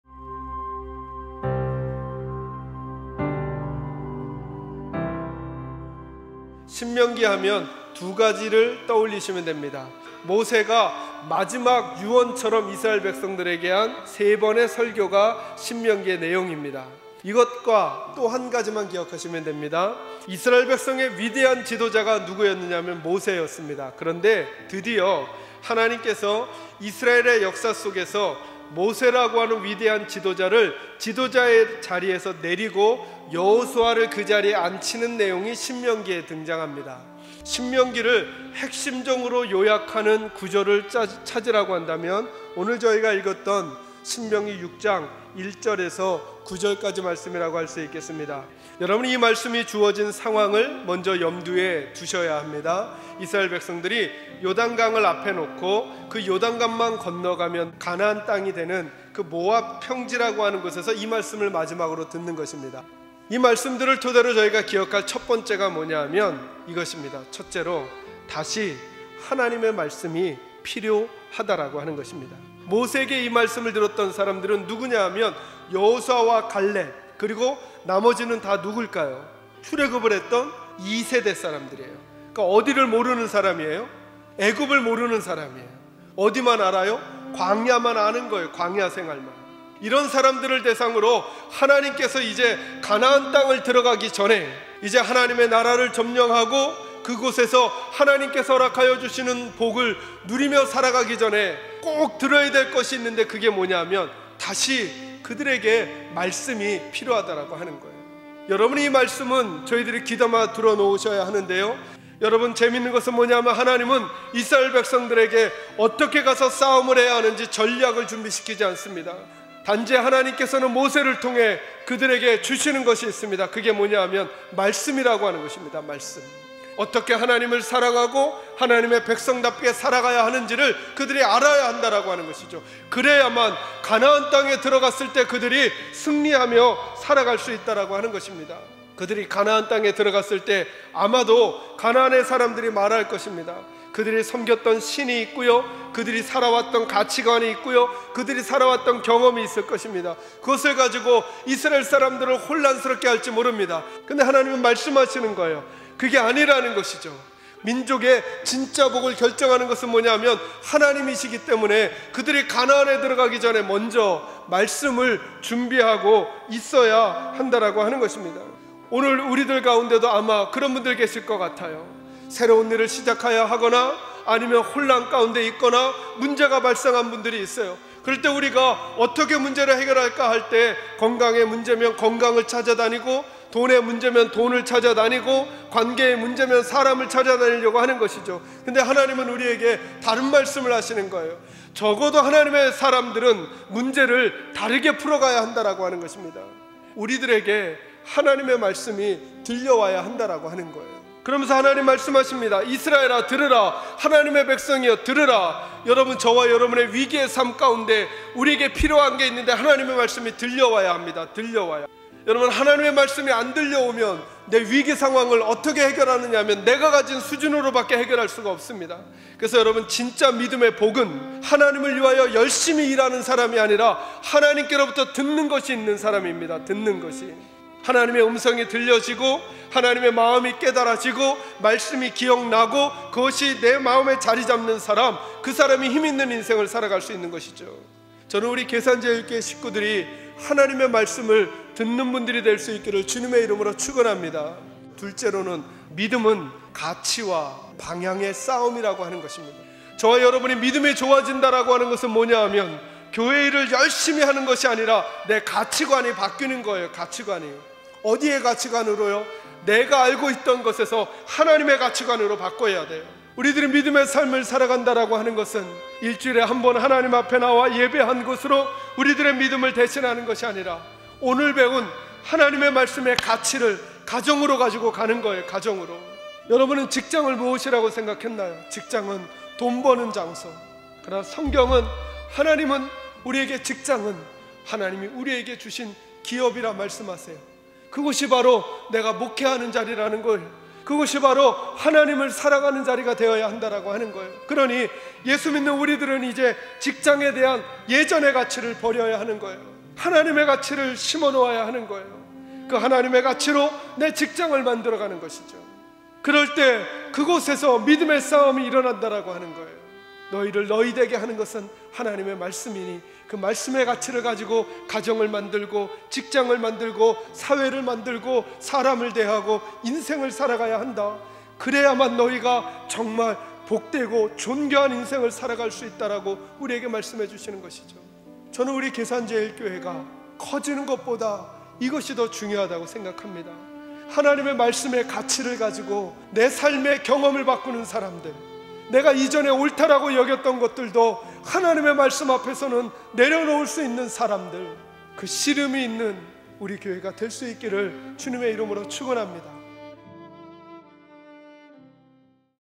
[행복한 5분 설교] 성경과 함께하는 행복한 여행(5)_신명기_말씀으로 세워가는 하나님의 백성 (신 6:1~9) 권오규 담임목사
본   문 : 신명기 6장 1~9절
제   목 : 성경과 함께하는 행복한 여행(5)
             신명기_말씀으로 세워가는 하나님의 백성
             권오규 담임목사